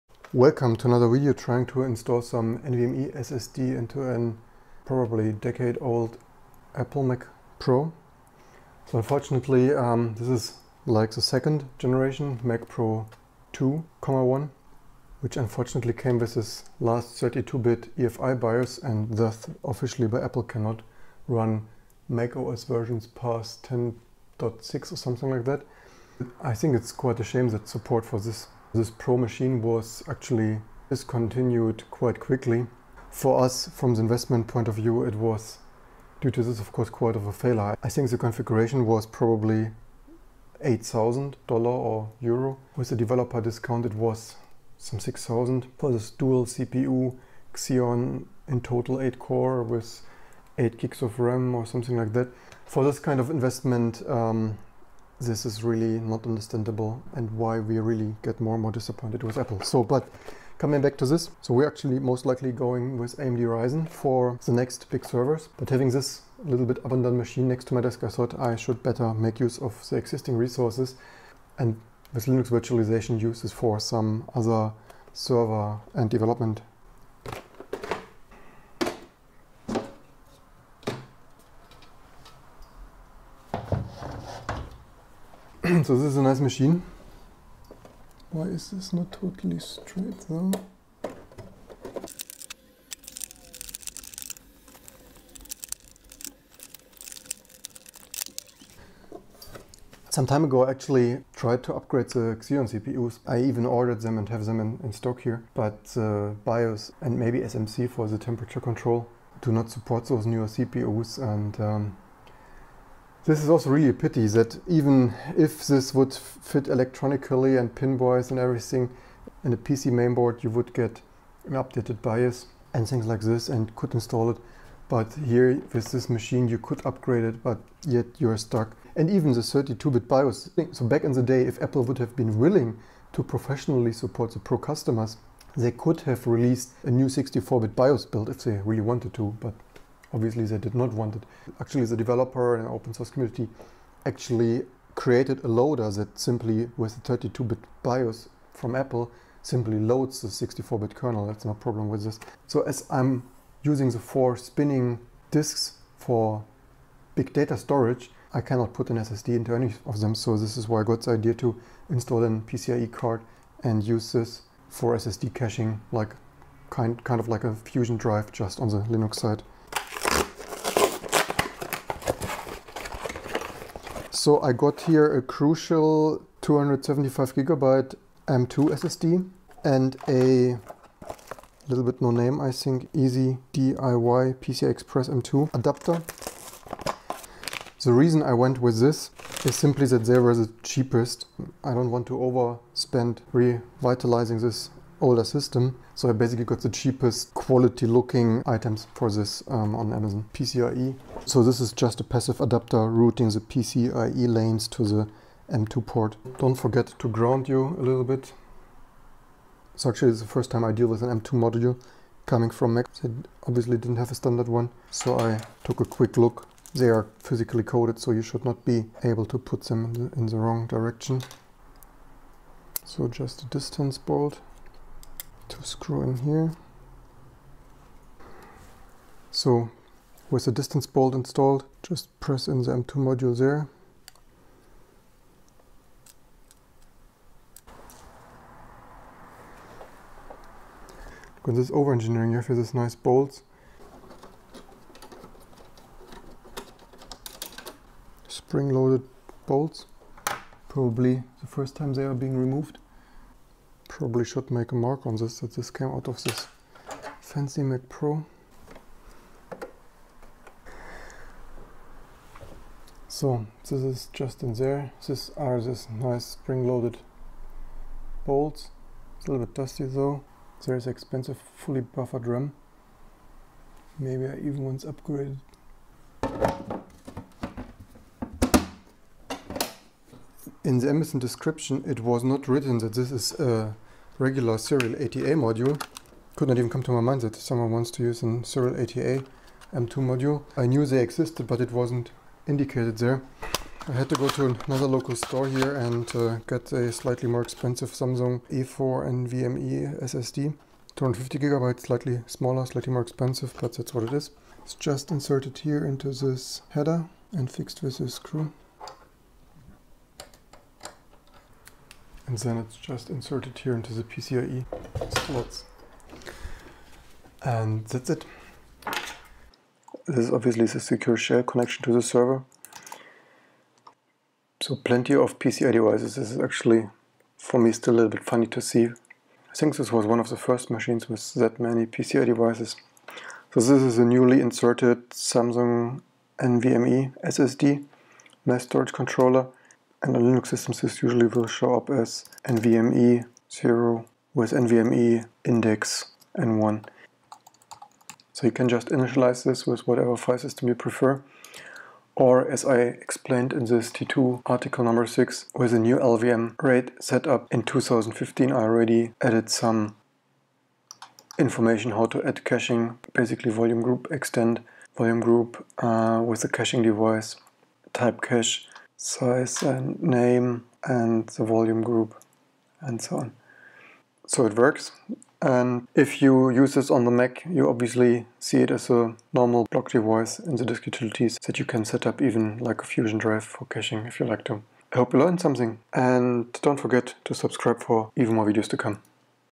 Upgrading classic MacPro w/ PCIe NVMe SSD, plus Linux details & Apple rant
Supercharging good old classic MacPro by installing a PCIe NVMe SSD for vastly improved I/O performance. Ad: Samsung NVMe M.2 SSD @Amazon